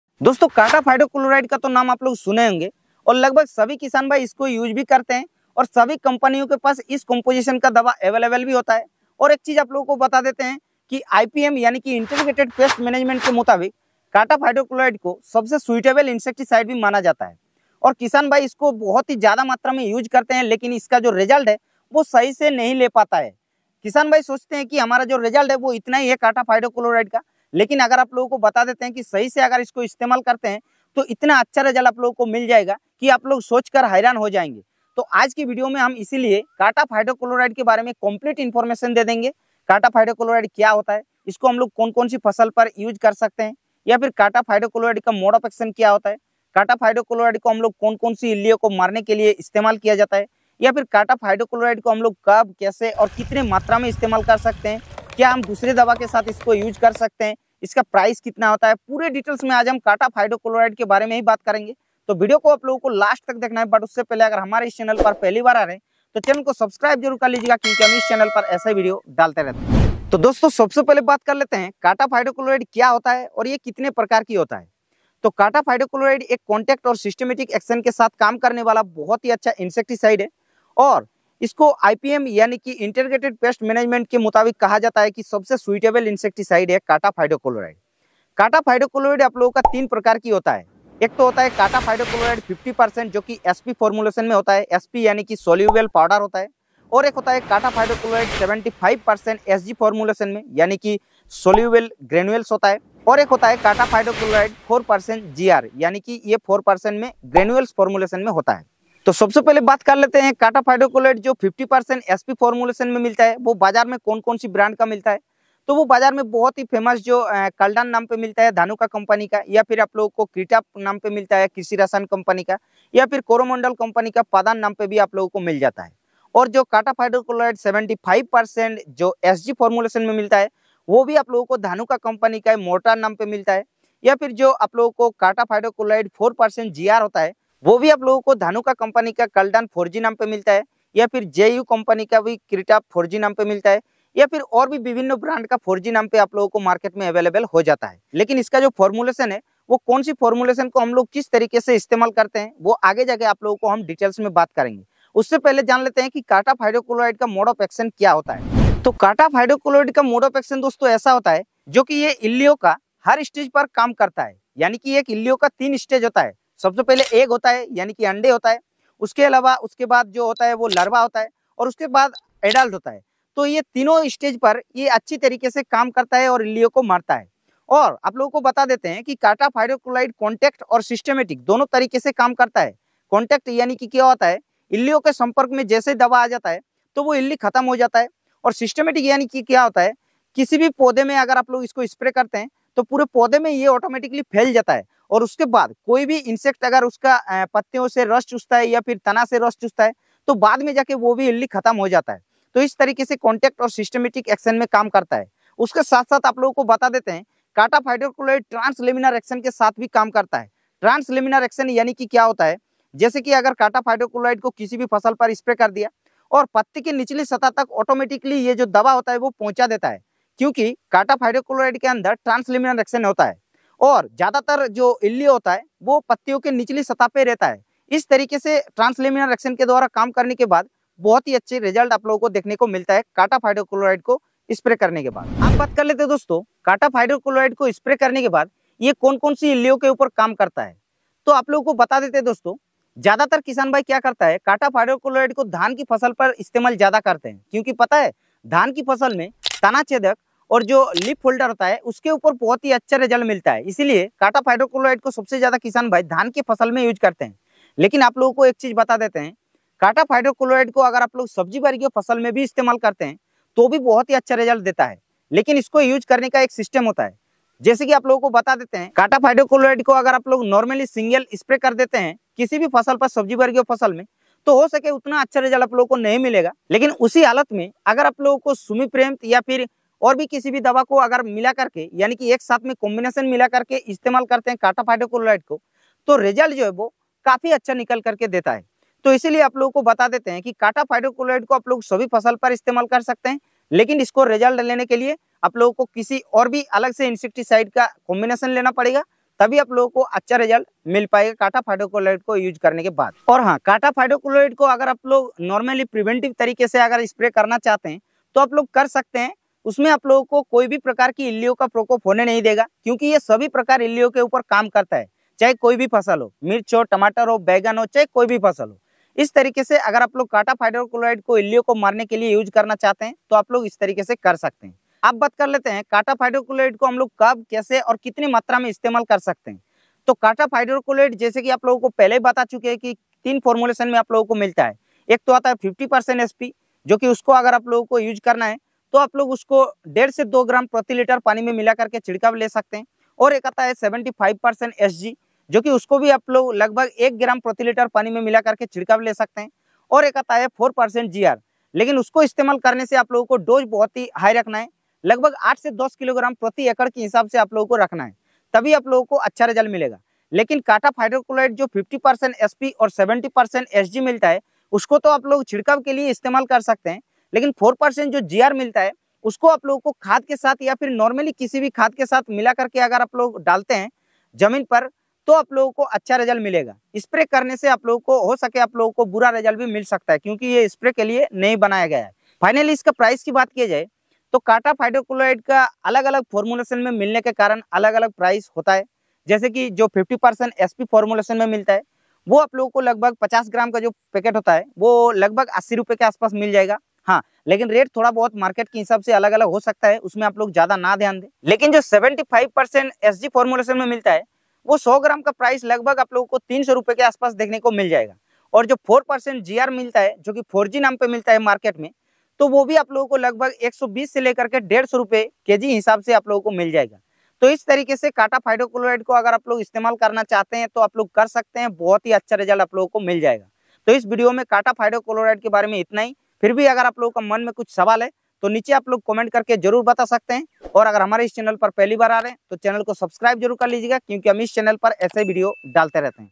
Cartap Hydrochloride का इतना दम 🐛🪲
I am Biplab Kumar Wellcome To My YouTube Channel Agri Duniya.

About This Video :
Doston is video mein humne Cartap Hydrochloride insecticide ke bare mein puri details mein information diya hua hai jaise ki Cartap Hydrochloride kya ha kaun kaun si crop mein spray kar sakte hain uske alaba iska benefits kya ha aur iska doge kitna hai. asha karta hun video aap logon ko achcha lagega agar achha laga to like kar dena.